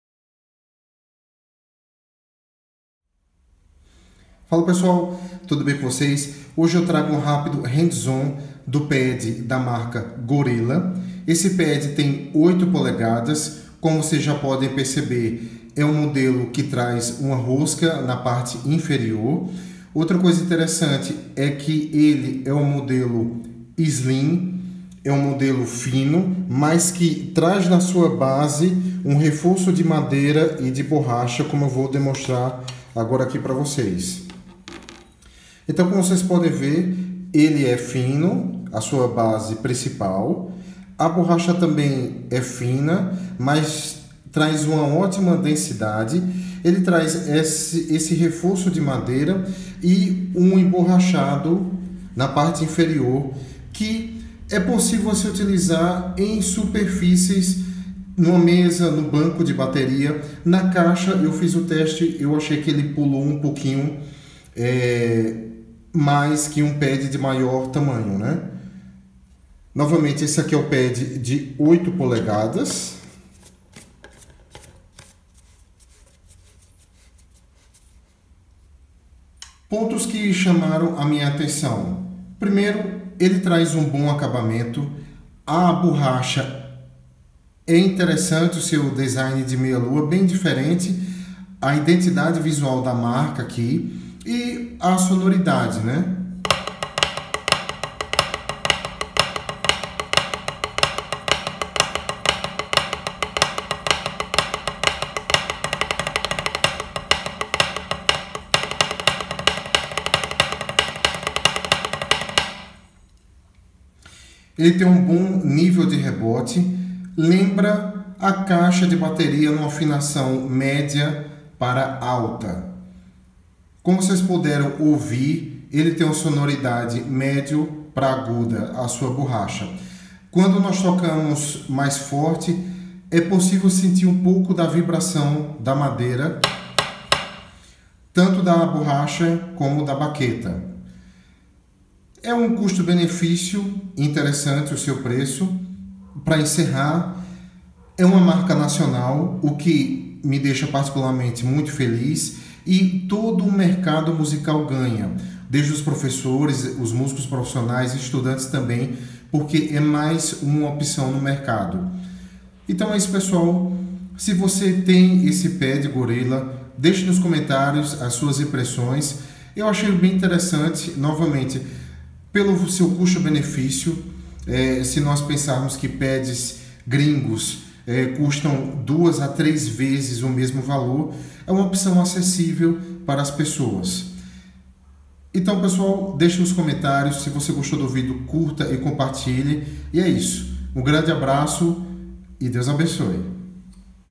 HANDS ON! Pad de Estudo Gorilla 8" PGR8 com Rosca Inferior
Hoje trago um rápido handson do Pad de Estudo Gorilla Clássico 8" PGR8 com Rosca Inferior.
Um pad com um bom custo-benefício.